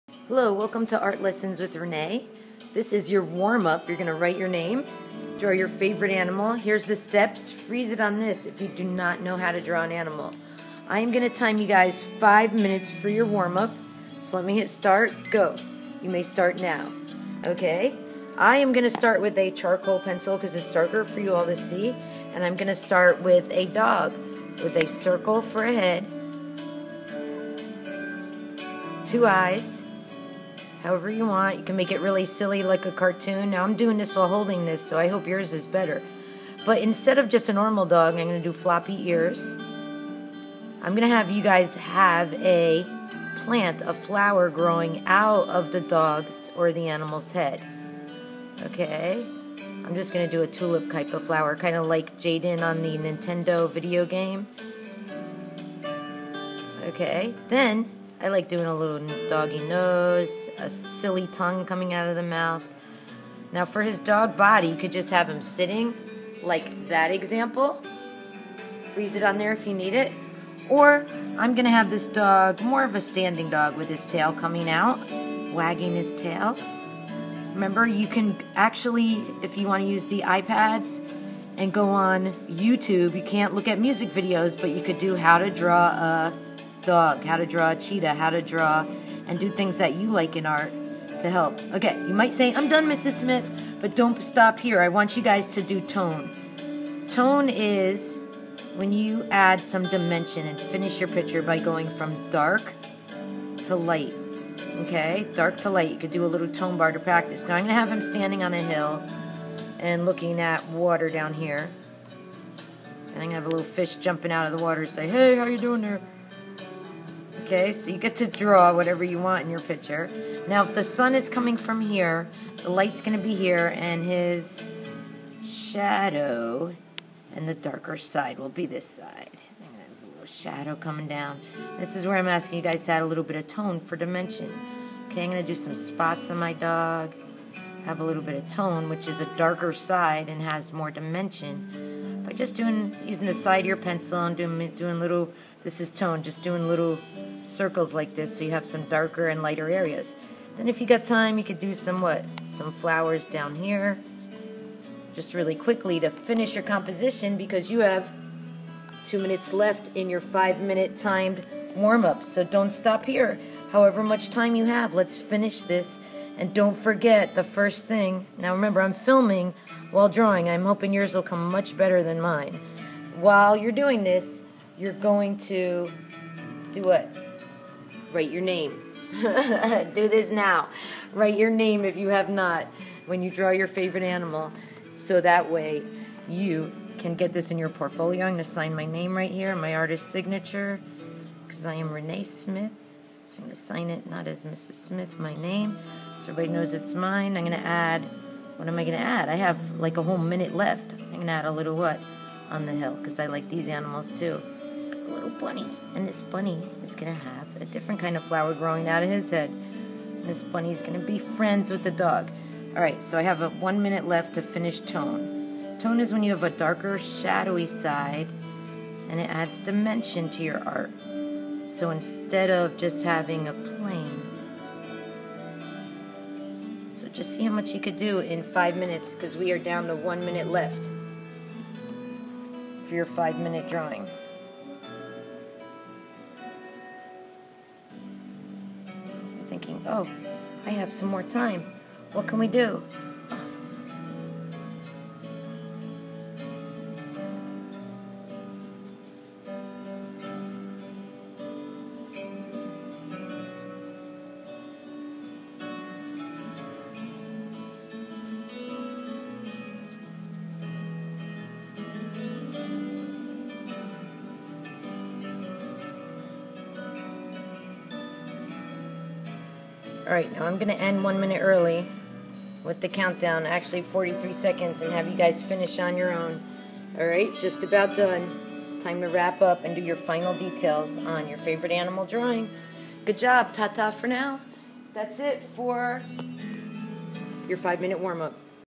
5 min. Drawing Warm-up, animal flower head
Draw your favorite animal with a flower growing out of his head! Go!